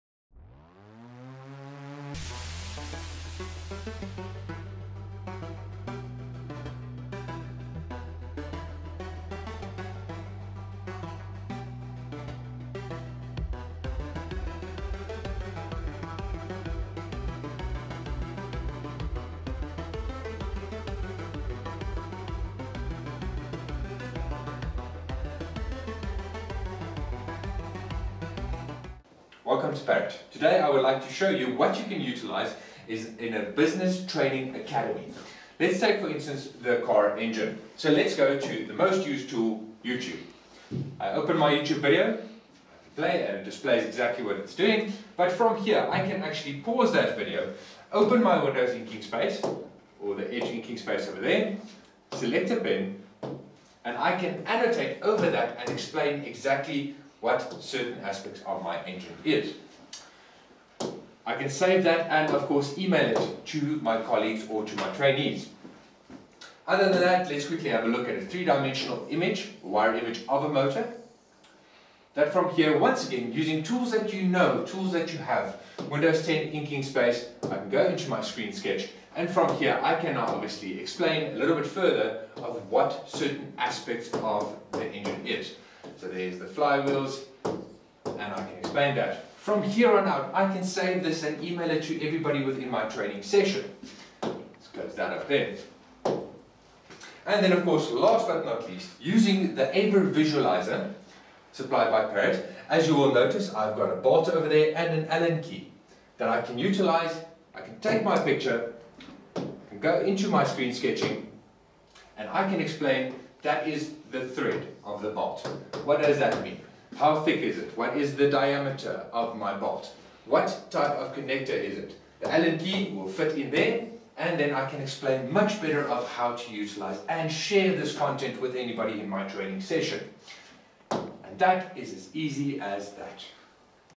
Parrot Products (Pty) Ltd - Mounting a Parrot LED Panel and using it in a Corporate Environment
We are going to show you how to mount a Parrot Interactive LED Touch Panel and how it can be utilized in a Corporate Environment and how to use a Visualiser.